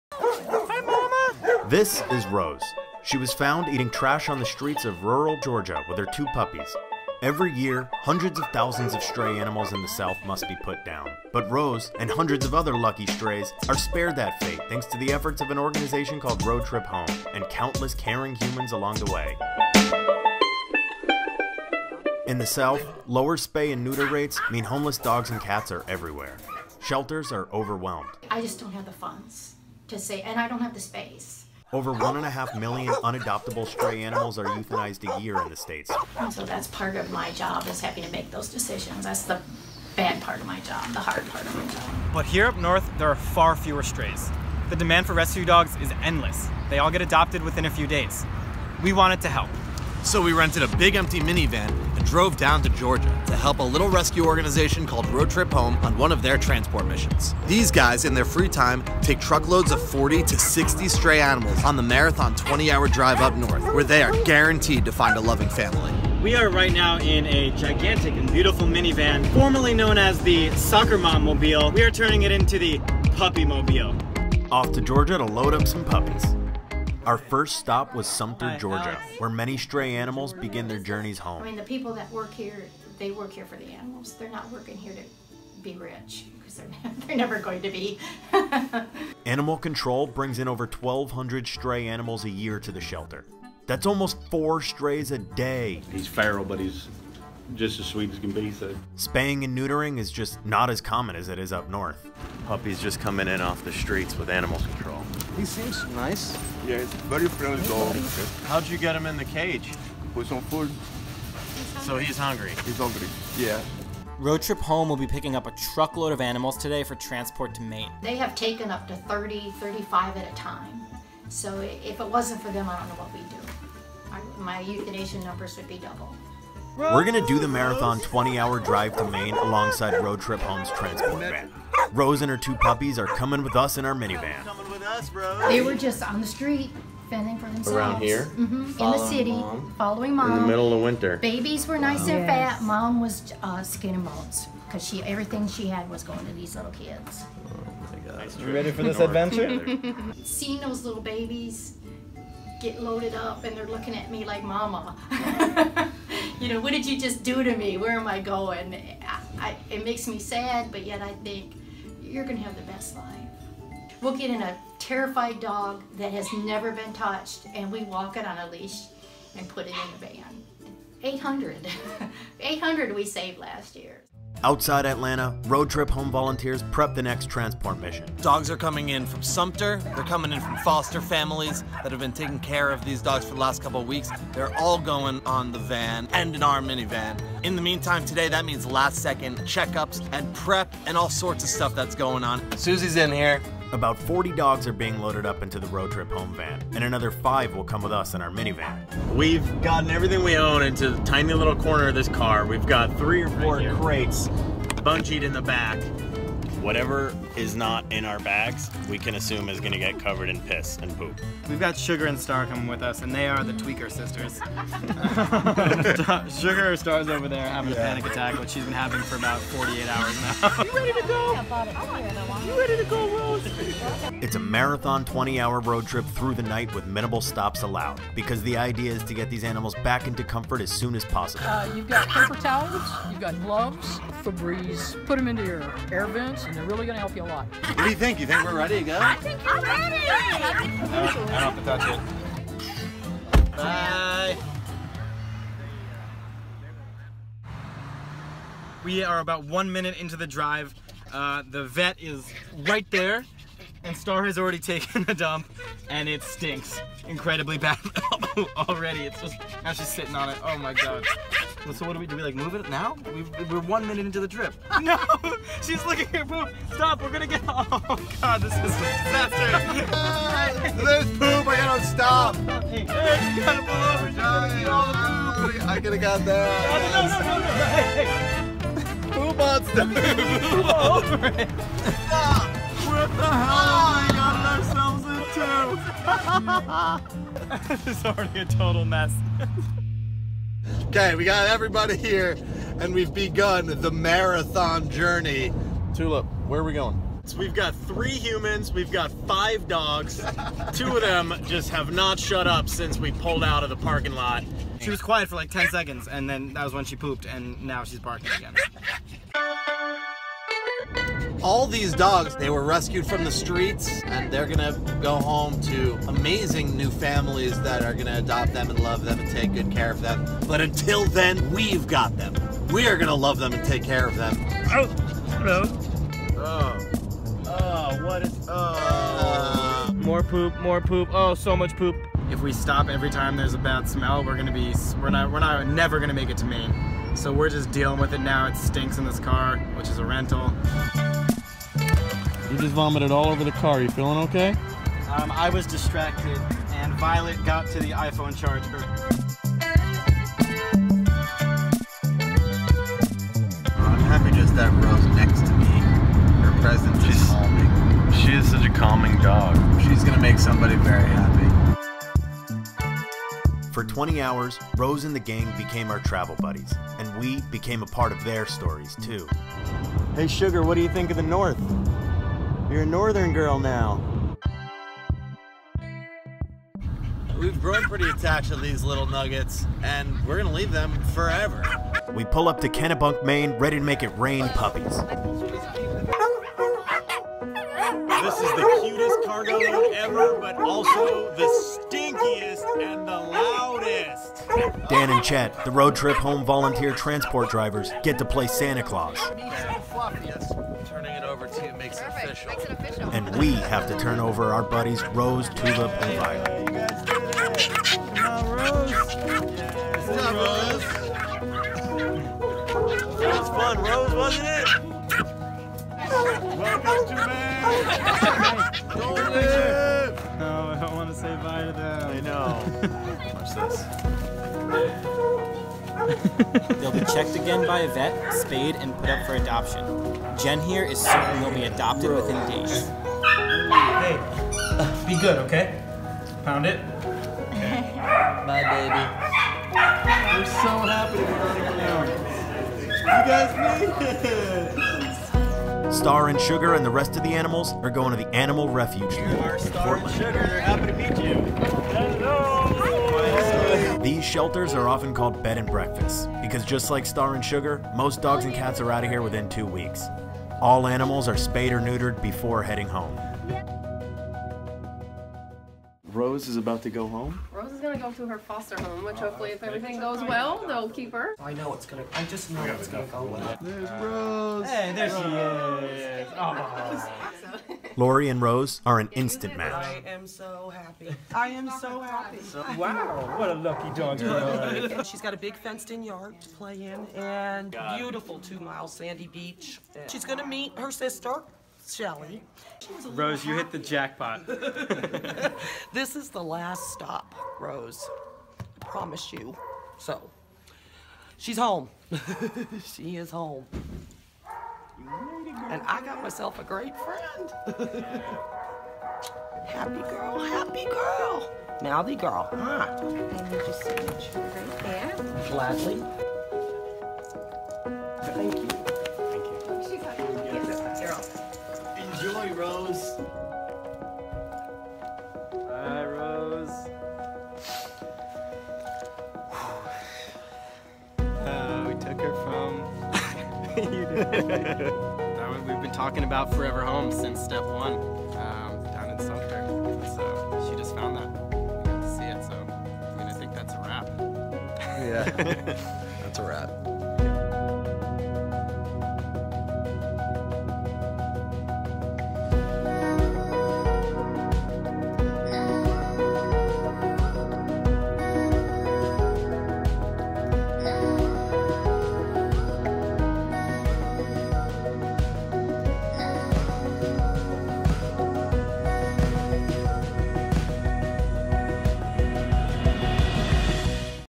How do you RESCUE 1,000+ STRAY DOGS a year??
In the South, there's an overload of stray cats and dogs. With underfunded shelters, what can you do to save their lives and make sure they find a home?

Watch to find out how we helped get Rose rescued!

We worked with some incredible animal rescue organizations to make this video:

- Road Trip Home Animal Rescue saves over 1,000 homeless animals
a year by driving them up north
- Sumter HumaneSociety works round the clock in rural Georgia to care for animals brought in off the streets
- Animal Welfare Society and Animal Refuge League of Greater Portland are state of the art shelters in Maine that help these dogs find great forever homes

Spay! Neuter! Foster! Rescue!